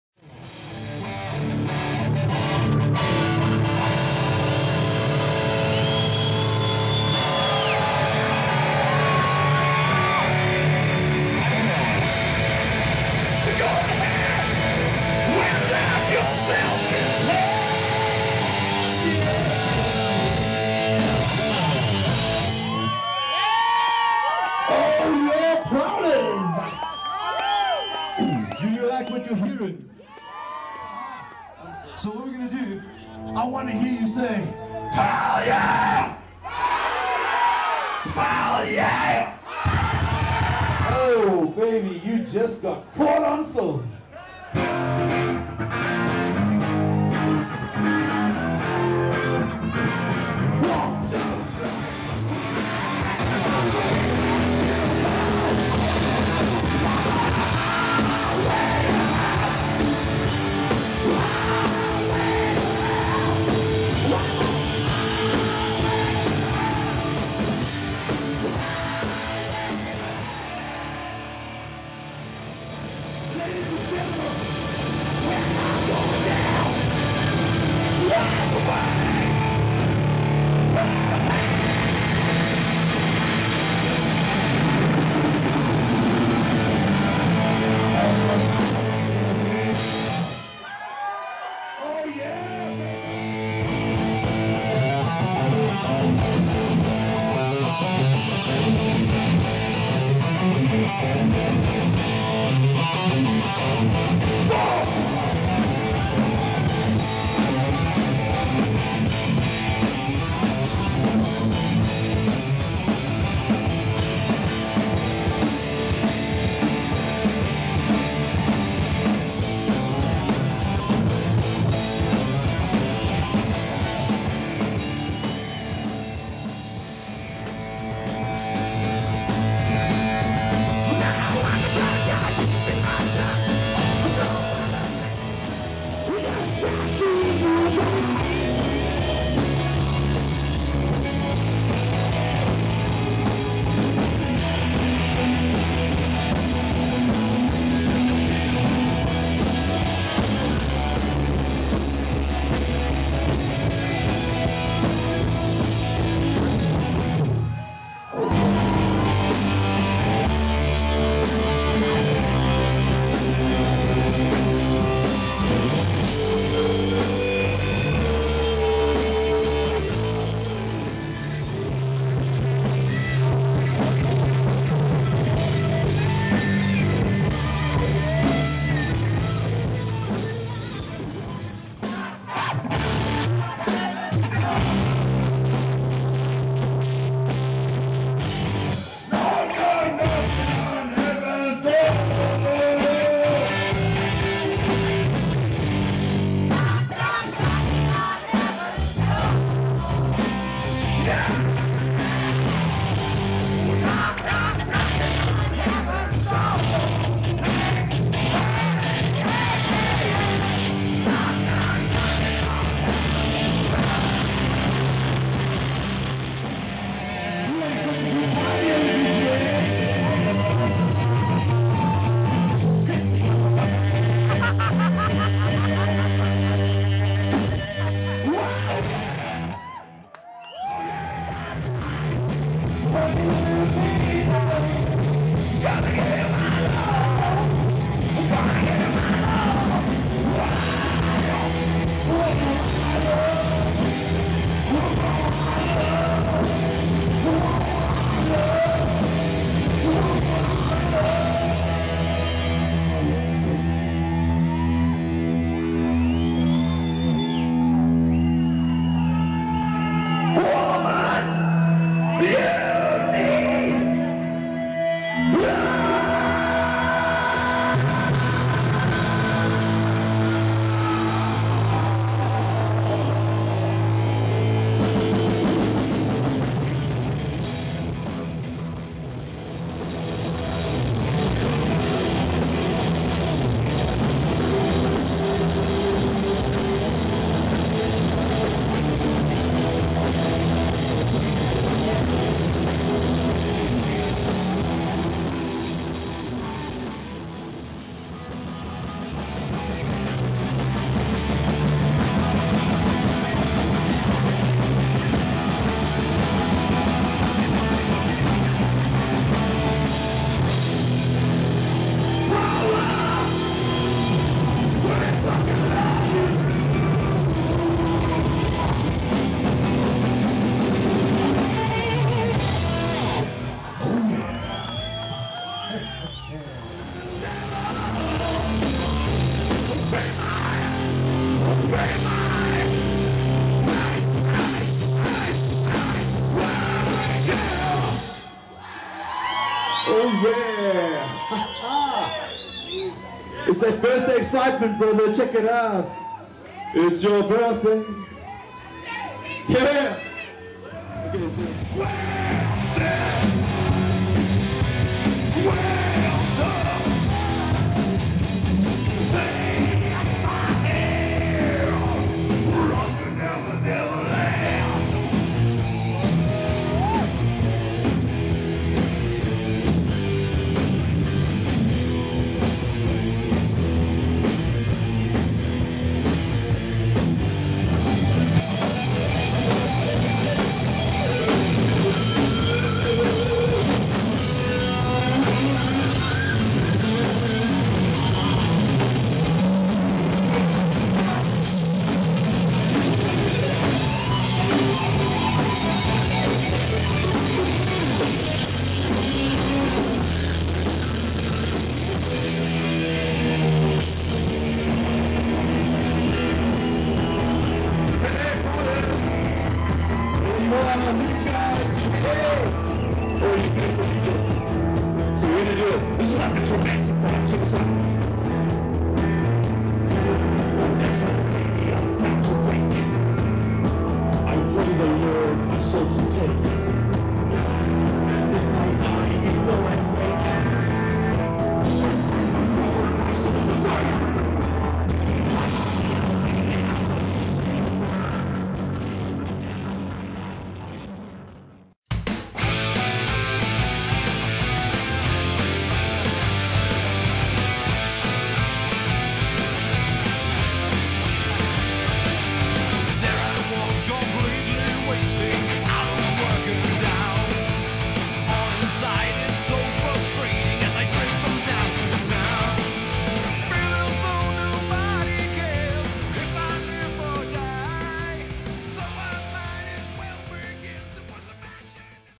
Live Burning
The Burnings live performance at the 2014 Prowlers XX B-day bash.